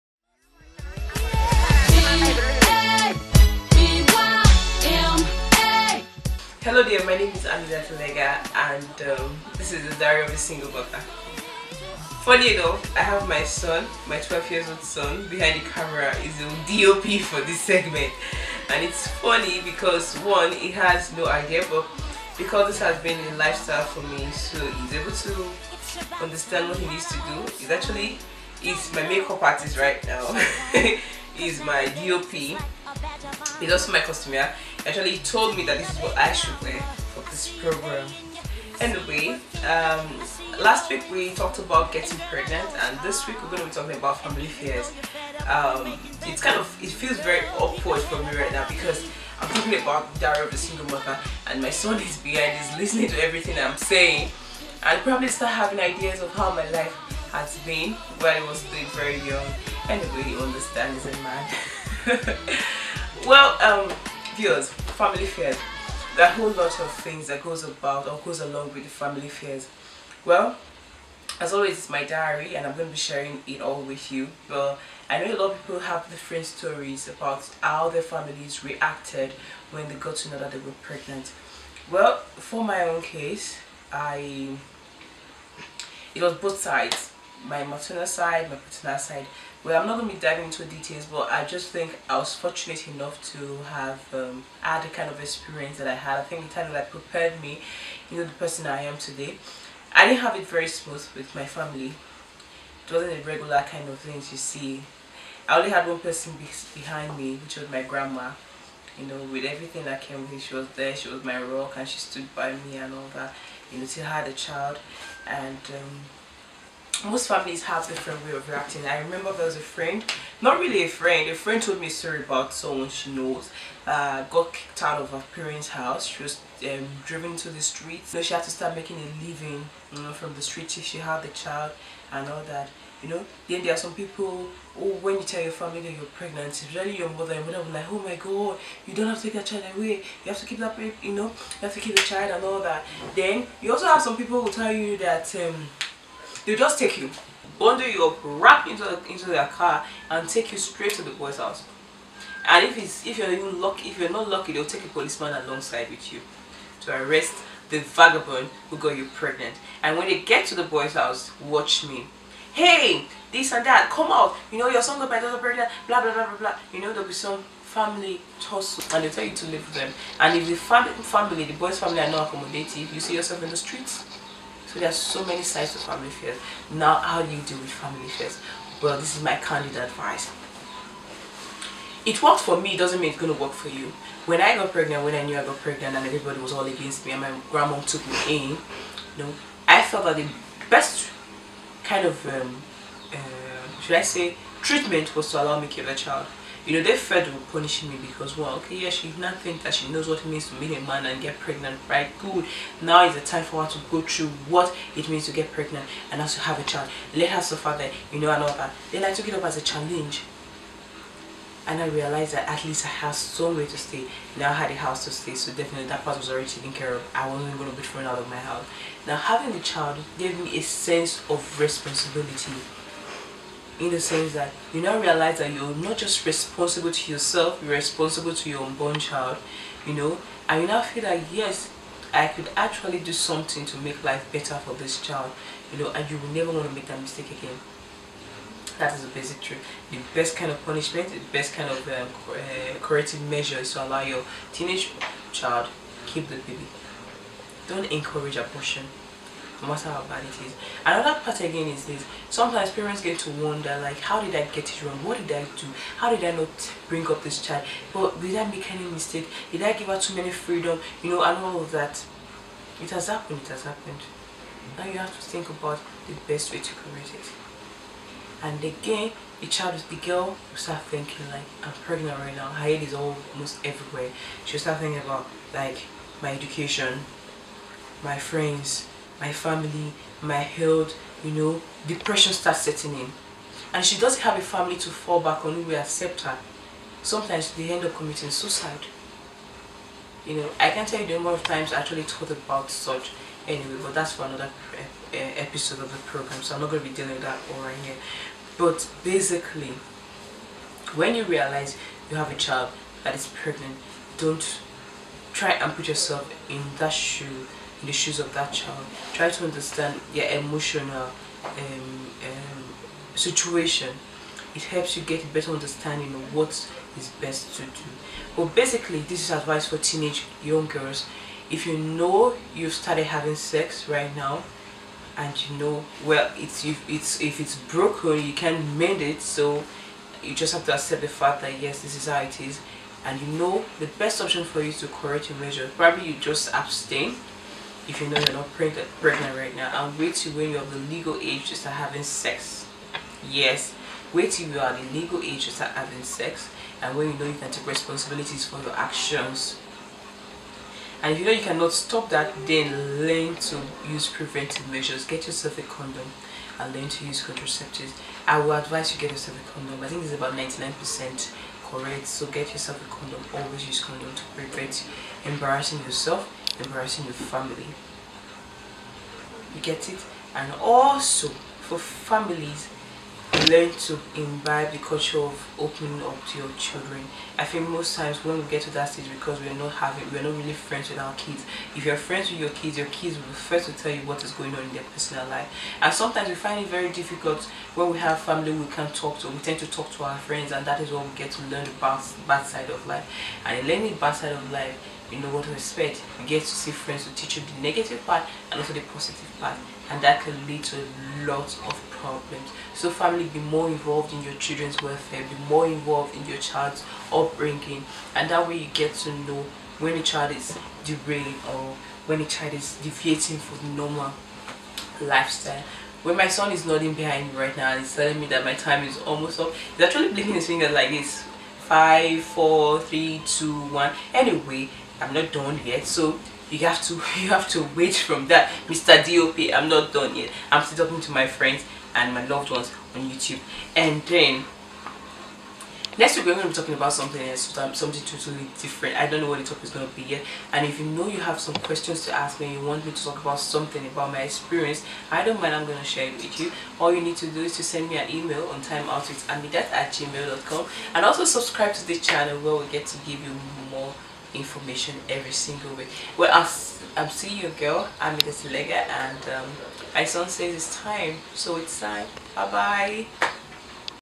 DIARY OF A SINGLE MOTHER S1E2 : FAMILY FEARS
This week I am Sharing my experience with you on how my family took the information about my pregnancy.